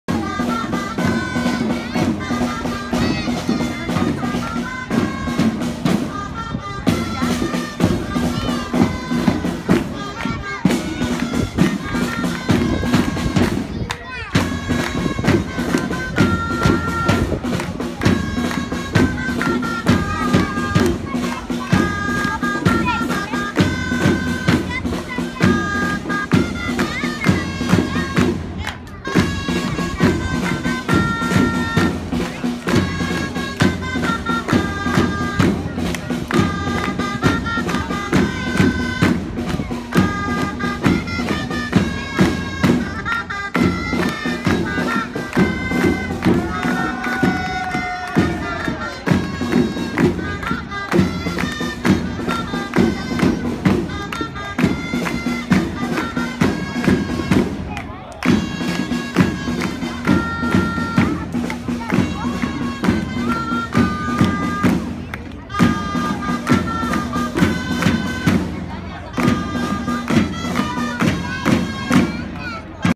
Pubilla i Sac i Ganxo de Sant Feliu de Codines
La Pubilla i el Saci Ganxo de Sant Feliu de Codines ballant a Bigues i Riells.